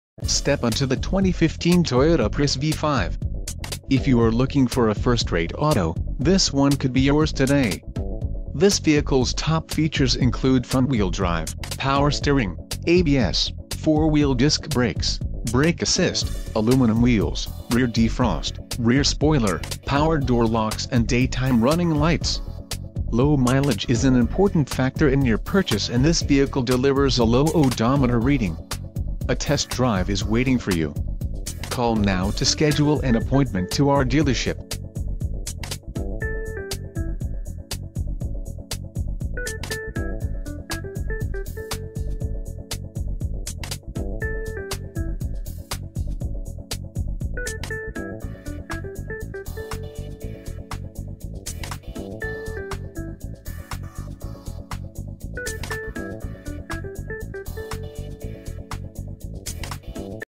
2015 Toyota Prius v Five - Stadium Toyota - Tampa, FL 33614
New Arrival! -Popular Color- -Great Gas Mileage- *Navigation* This 2015 Toyota Prius v Five is Attitude Black with a Bisque interior which is a very popular and great looking color combination.  This Five comes with great features including: Navigation System, Satellite Radio, Keyless Entry, and Tire Pressure Monitors . It is rated at 40.0 MPG on the highway which is fantastic! Buy with confidence knowing Stadium Toyota Scion has been exceeding customer expectations for many years and will always provide customers with a great value!